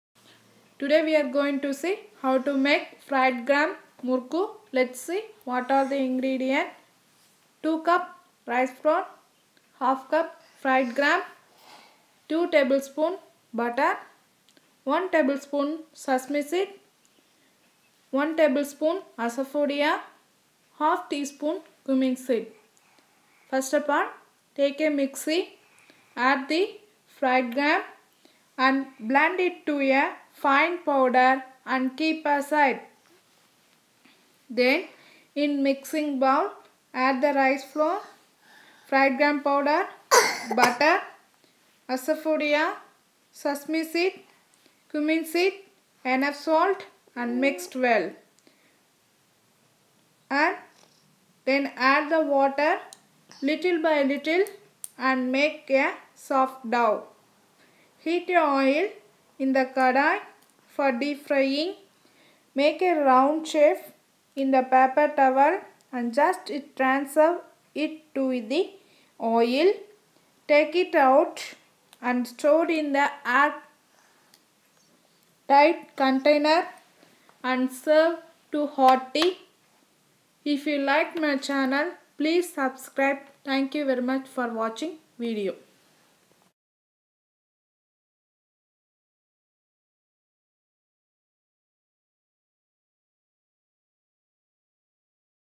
Pottukadalai murukku or split roasted gram murukku or fried gram murukku
fried gram murukku-how to make split roasted gram murukku or fried gram murukku
INGREDIENT
Oil for frying
Rice flour-2cups
Fried gram-1/2 cup
Asafoetida-1 tsp
Cumin-1/2tsp
Sesame seed-1 tsp
Butter-2tsp
Salt